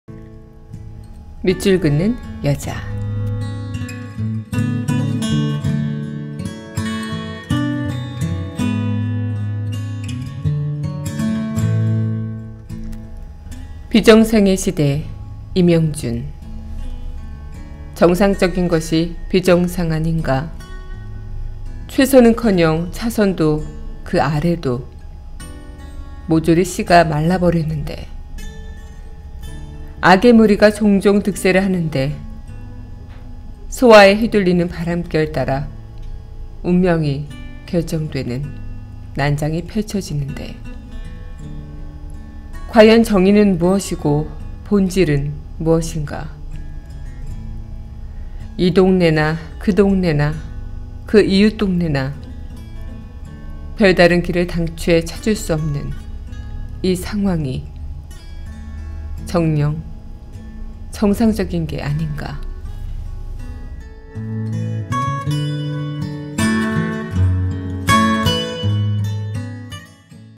[밑줄긋는여자] 비정상의 시대 - 임영준
11월 11일 문화다락방 - 밑줄긋는여자에서 강민선 아나운서의 시 낭송으로 임영준 詩 '비정상의 시대'를 만나보시겠습니다.

비정상의 시대 - 임영준

정상적인 것이
비정상 아닌가
최선은커녕
차선도 그 아래도
모조리 씨가
말라버렸는데
악의 무리가 종종
득세를 하는데
소아에 휘둘리는
바람결따라
운명이 결정되는
난장이 펼쳐지는데
과연 정의는 무엇이고
본질은 무엇인가
이 동네나 그 동네나
그 이웃 동네나
별 다른 길을
당최 찾을 수 없는
이 상황이 정녕
정상적인 게 아닌가